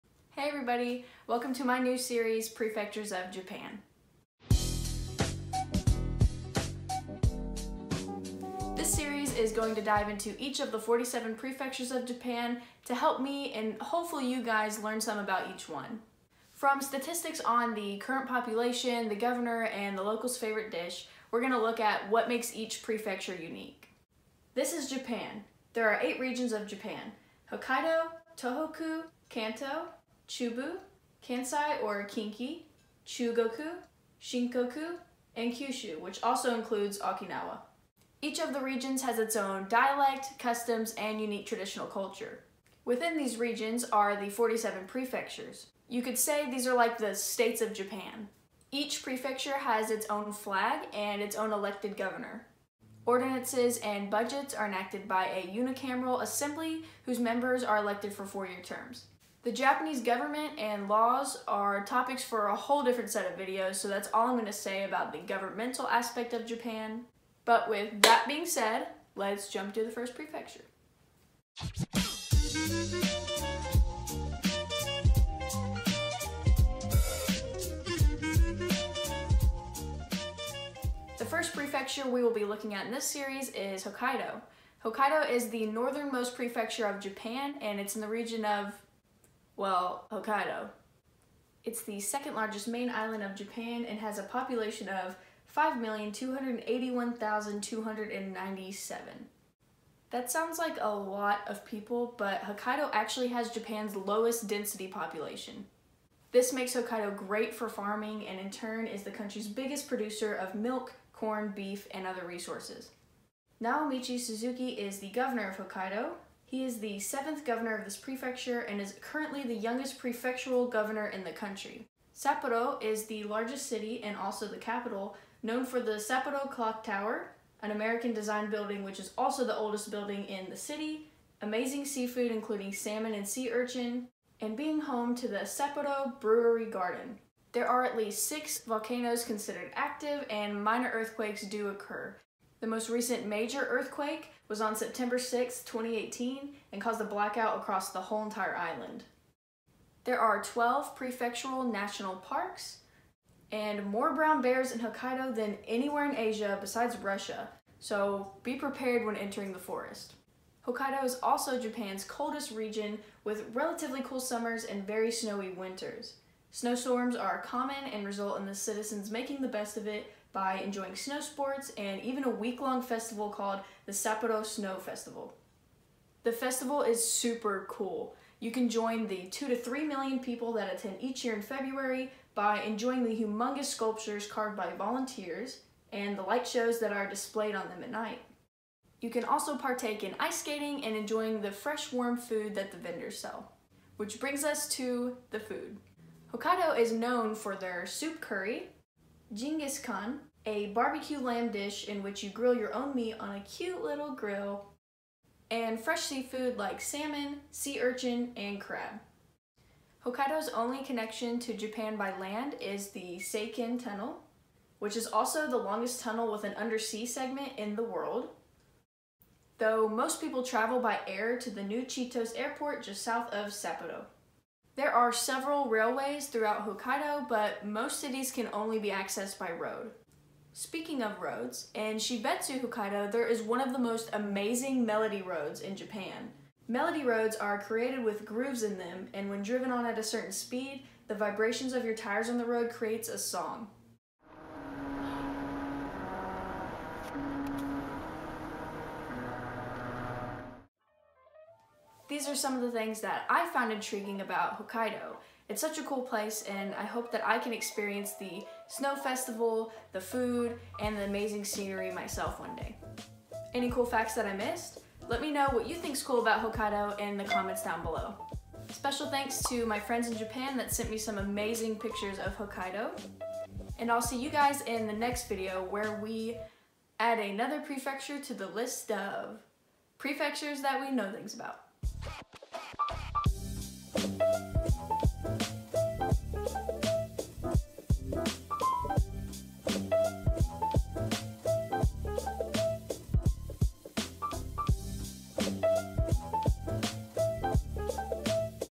Prefectures of Japan EP 1 - All About Hokkaido!
The Prefecture of Hokkaido!
This video is the first of the series: Prefectures of Japan
Learn some facts about the ❄️coolest❄️ prefecture in the country.

🔔Turn on bell notifications to see new videos as soon as they’re posted!🔔